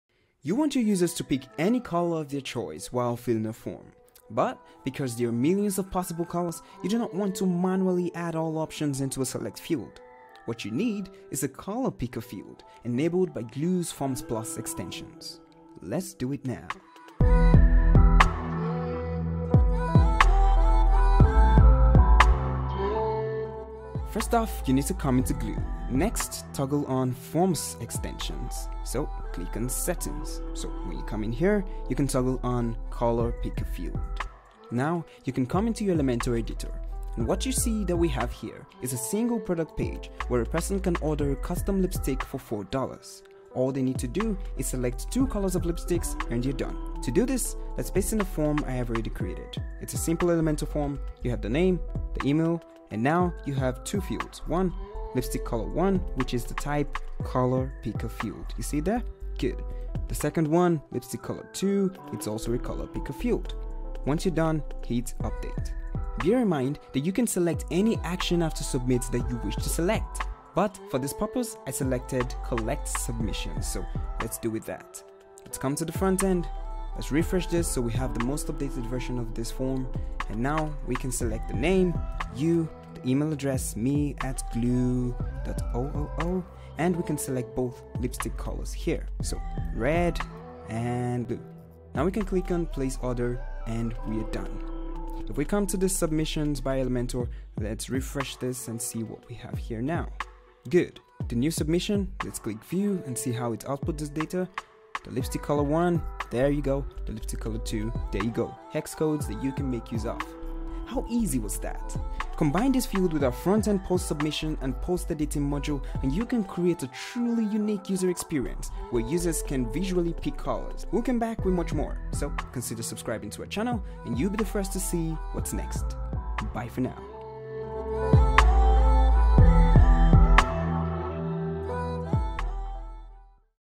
Color Picker Field - SuperGloo Forms Extensions+ - Tutorial
Gloo for Elementor - Forms Extensions+ - Color Picker Field is an Add-on for Elementor Forms that will allow your users to select and submit as many colors as needed. In this video tutorial, you will learn how to use this powerful tool to reach the maximum potential for your website forms.

Color Picker Field adds a much-needed way for your users to submit colors through forms. This is extremely handy when, for example, choosing a custom color for a product they may be buying, or even customize sections of a post they're submitting thanks to the aforementioned Frontend Post Submission and Editing.

Form Extensions+ Color Picker Field is only one of the exciting new Gloo addons to take your website to the next level.